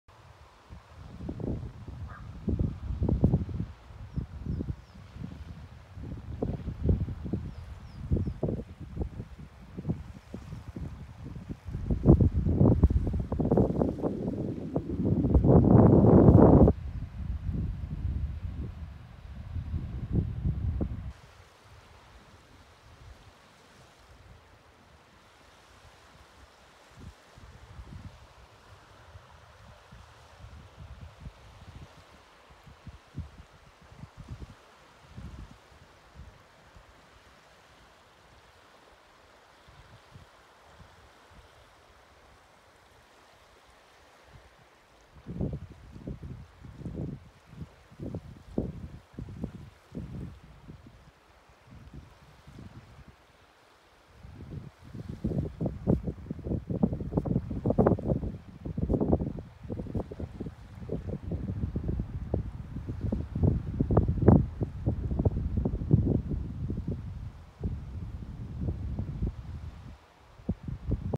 I stopped on the road to the Connemara National Park
I love Ireland and her harsh beauties. I was lucky it was such a glorious day.